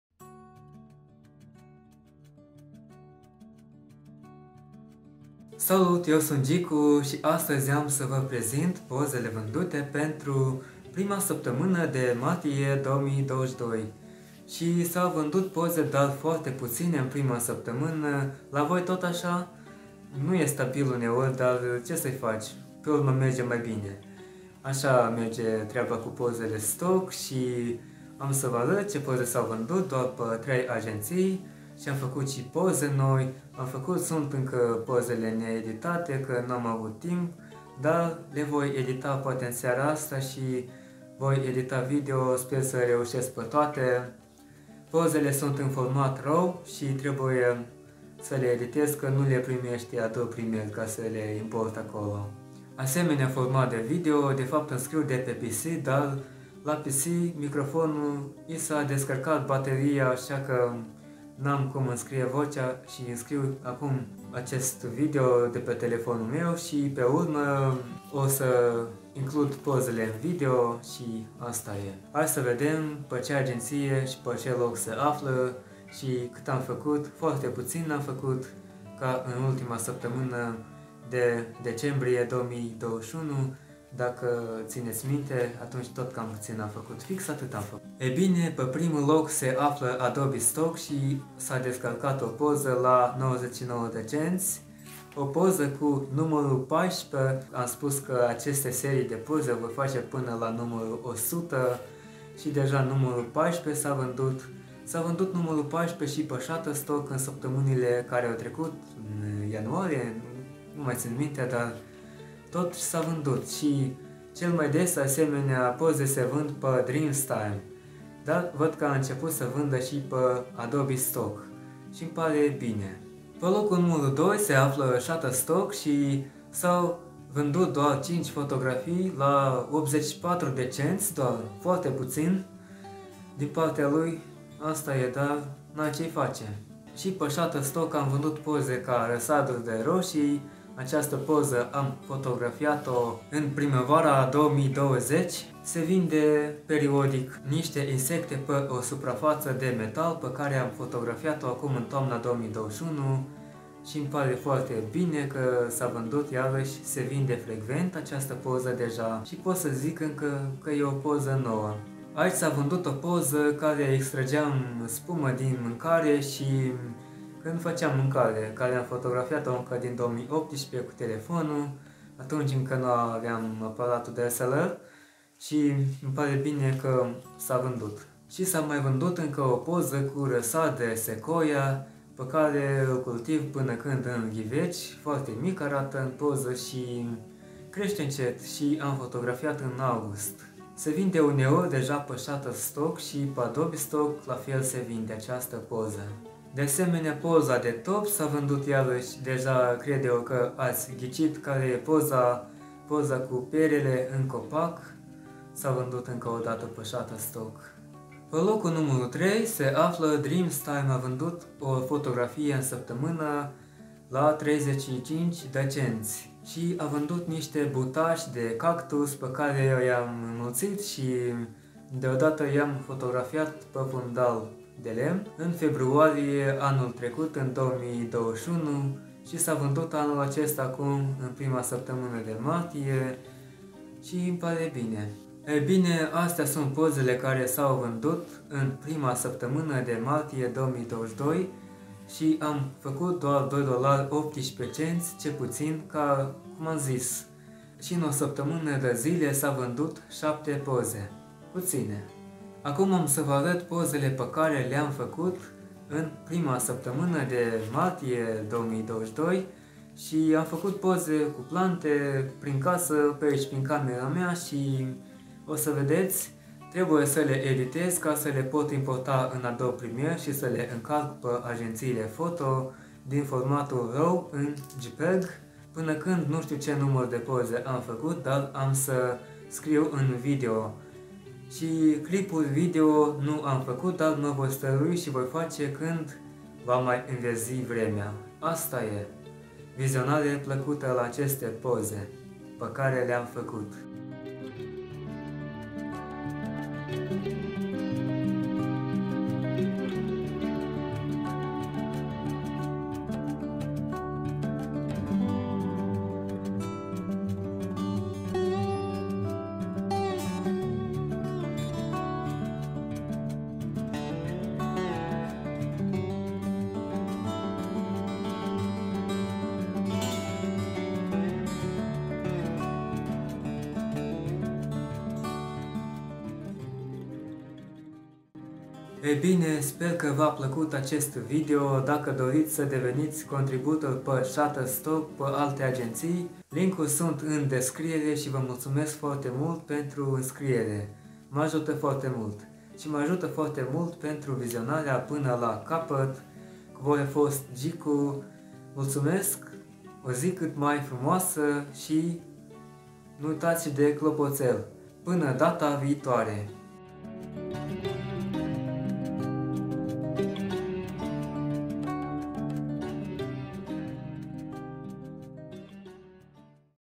Poze vandute in prima saptamana de Martie 2022
Salut, Iata ca a trecut si prima saptamana de Martie 2022, Si in acest video va arat pozele care s-au vandut pe agentiile foto, a fost o saptamana nereusita si am vandut foarte putine poze. Multumesc pentru vizionare!